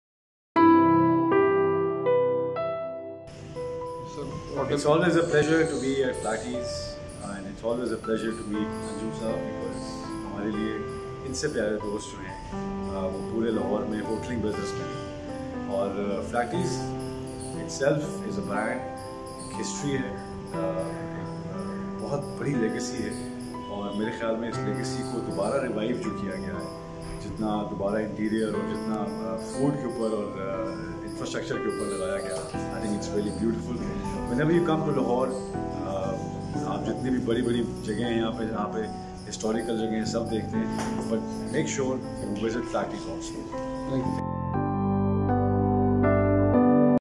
Shan Shahid
Director Operations Flattie's Mr. Irshad B Anjum warmly welcomes Filmstar Shan Shahid on his visit to Faletti's Hotel Lahore.